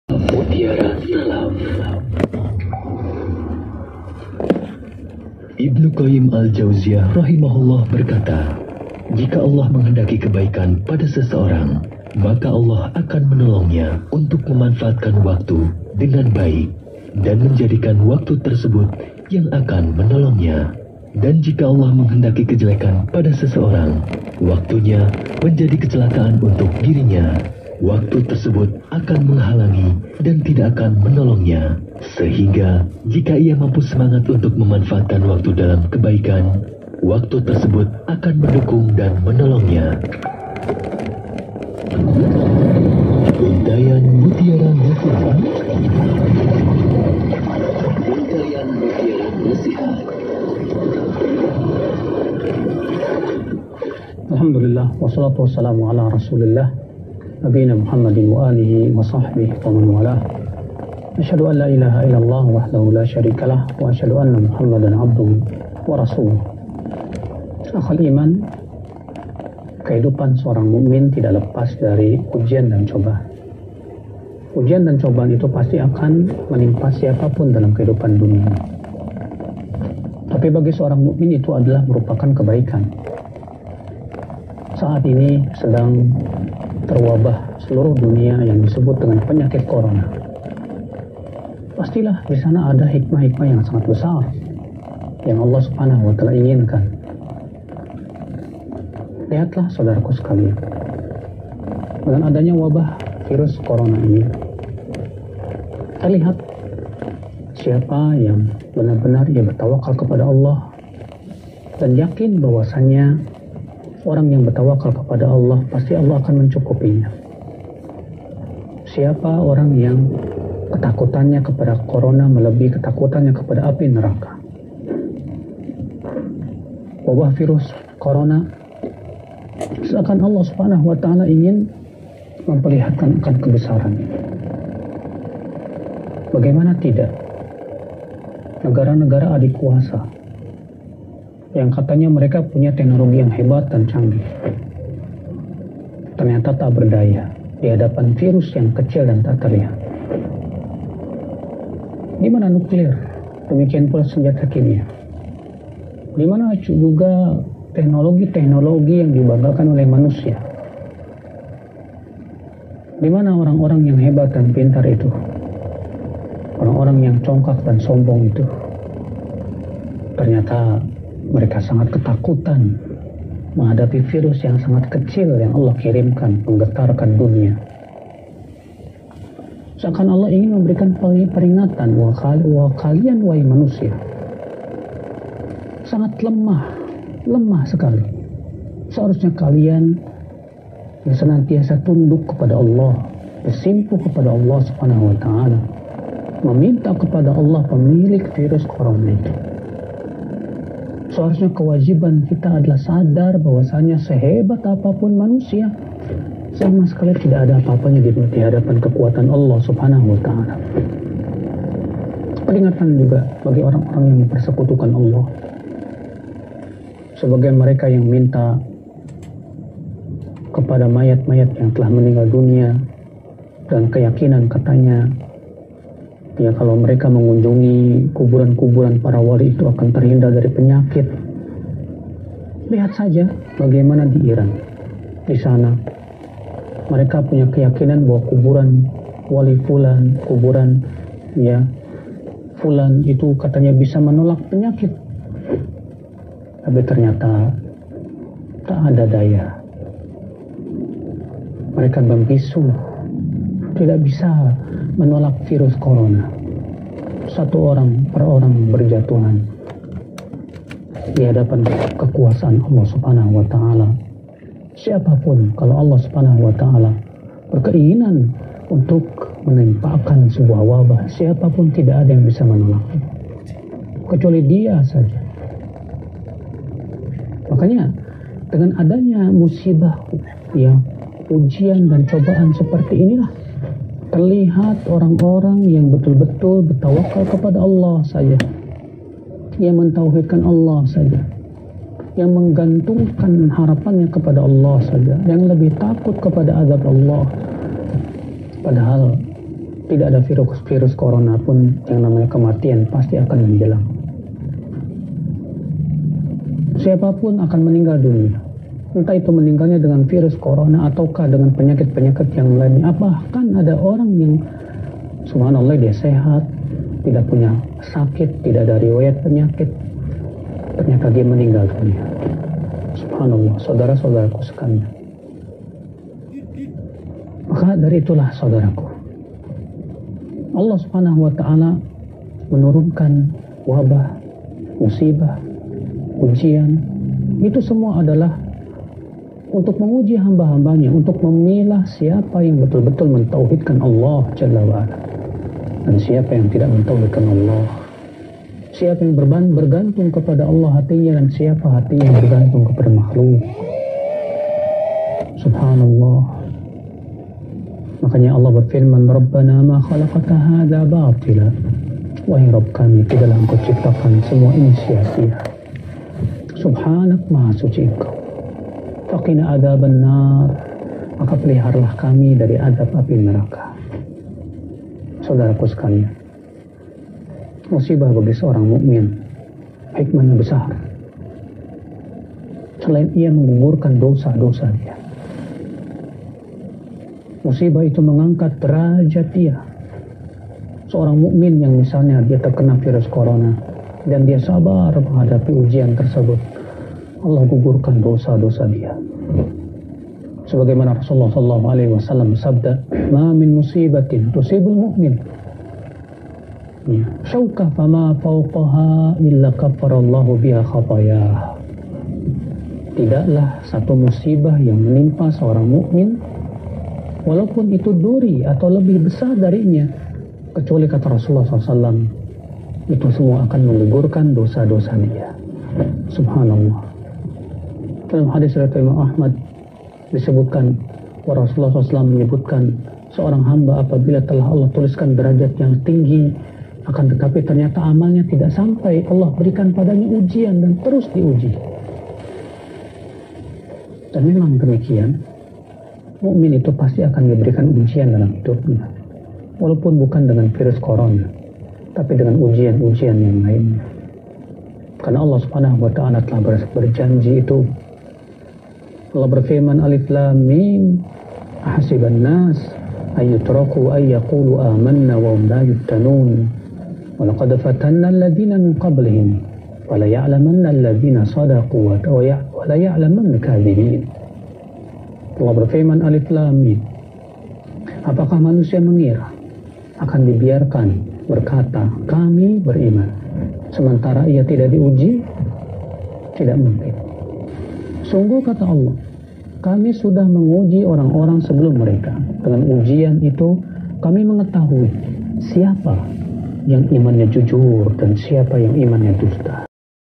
Mutiara nasehat, mutiara salaf || renungan waktu luang
Waktu luang bisa jadi bermanfaat bisa jadi bencana bagi kita. Dan berbagai ujian ketaqwaan akan dihadapi manusia.
Semoga video ini bermanfaat,merupakan rekaman dari siaran radio Rodja FM 100.1 Mhz.
Saya padukan dgn foto2 pulau padar labuan bajo Manggarai barat.
Semoga bermanfaat.
Salam bumbu world, bumbu dunia.